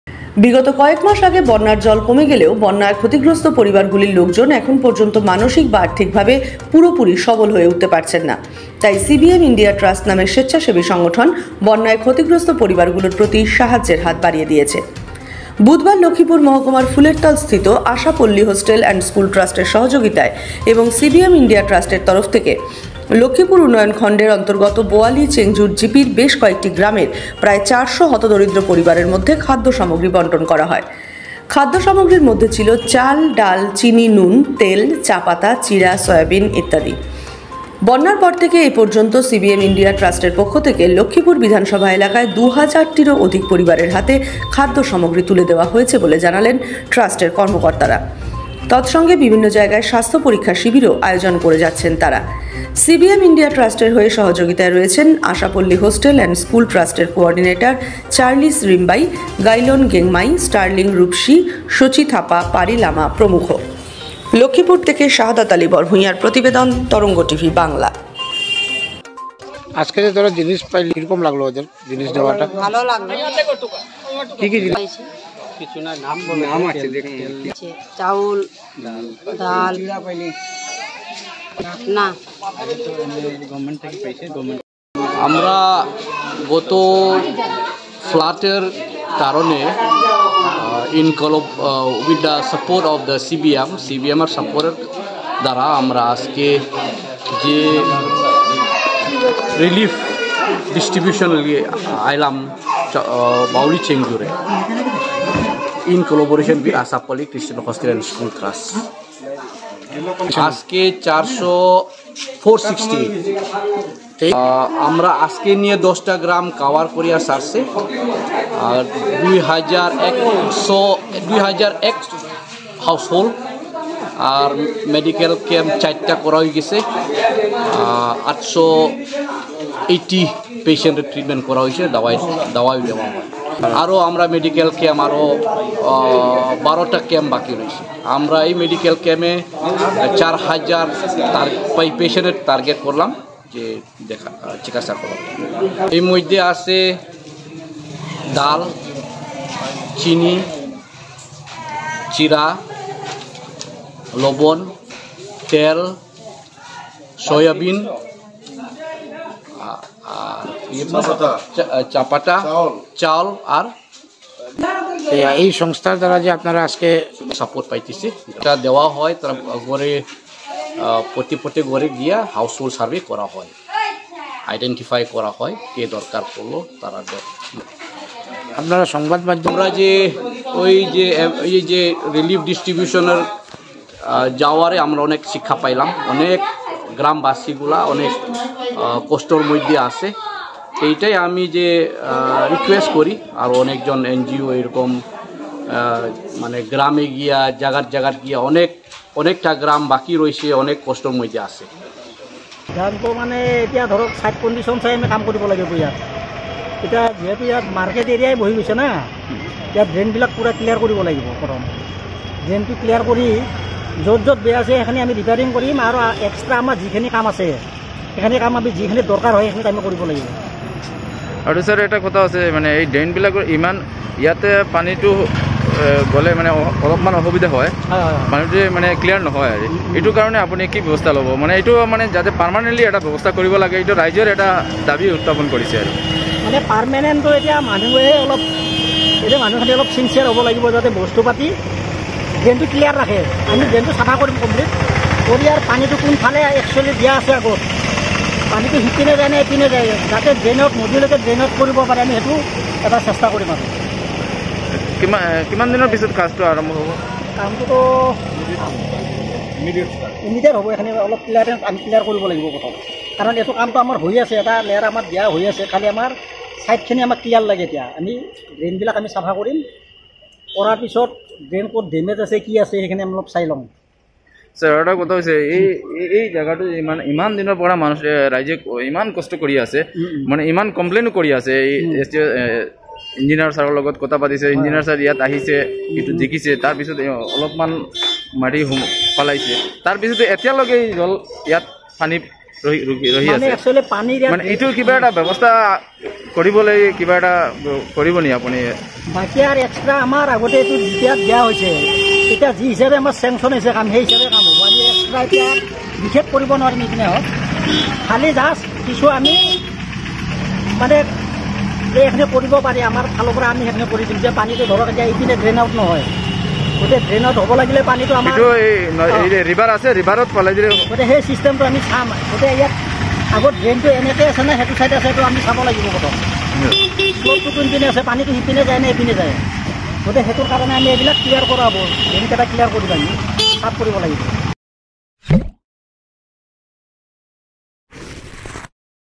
বন্যা ক্ষতিগ্রস্ত ৪০০ পরিবারের মধ্যে খাদ্য সামগ্রী বন্টন করলো সিবিএম ইন্ডিয়া ট্রাস্ট
leanings.